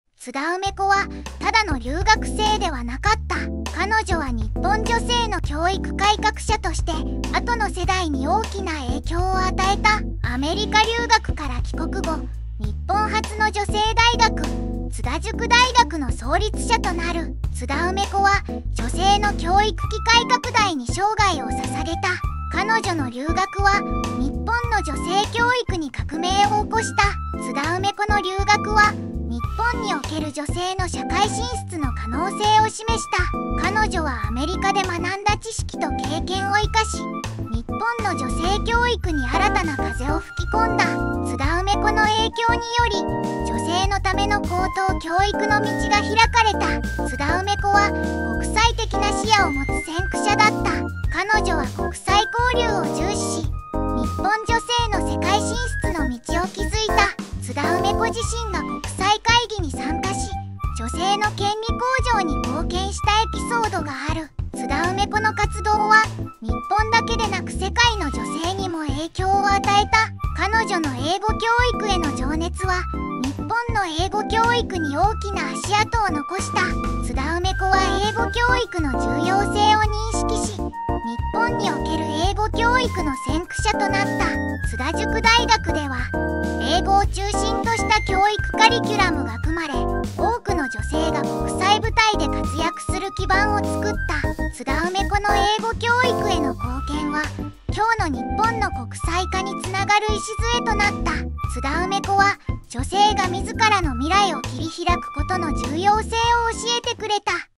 日本初の女性留学生、津田梅子の衝撃の真実5選！
津田梅子という偉大な人物が日本の女性教育にどのように革命をもたらしたか、その生涯を通じて彼女が達成した成果とその影響力に焦点を当てています。